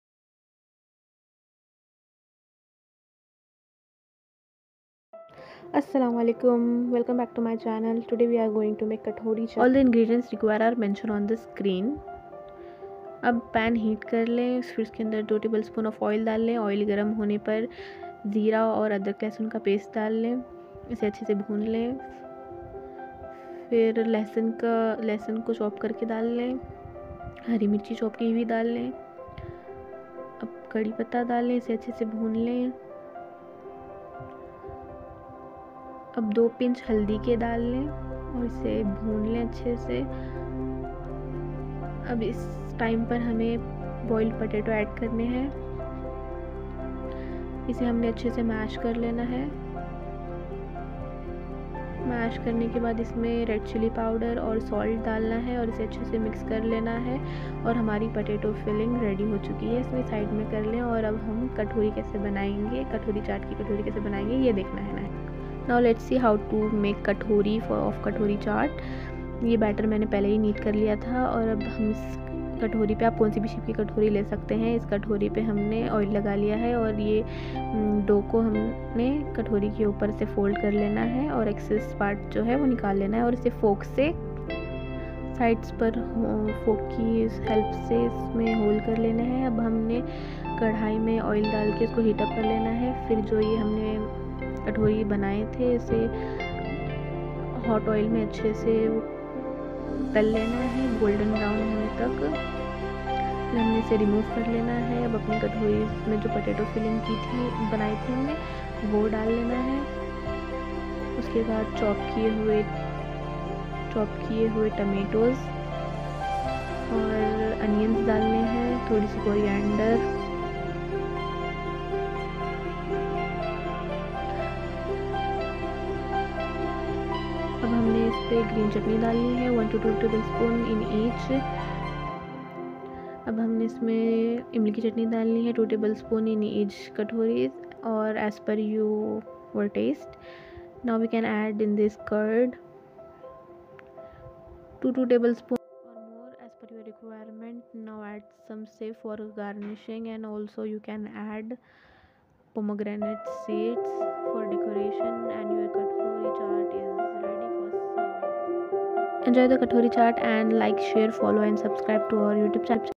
Chaat Lovers Rejoice:-Try This Simple and tasty 😋 Katori Chaat Recipe||Step-by-Step
In this video, we explore a unique and creative twist to the traditional chaat snack with our irresistibly delicious Katori Chaat recipe. With simple ingredients and easy steps, we demonstrate how you can make your own crispy edible bowls and fill them with a medley of tangy, savory, and sweet chaat flavors. Join us on this culinary adventure to impress your family and friends at parties or enjoy a delightful snack for yourself!Tamarind Chutney: A sweet and tangy sauce made from tamarind pulp, jaggery, and spices. It adds a burst of flavor to chaat dishes.

Mint Chutney: A refreshing and aromatic sauce made from fresh mint leaves, green chilies, lemon juice, and spices. It enhances the taste of chaat recipes.

Musician: @iksonmusic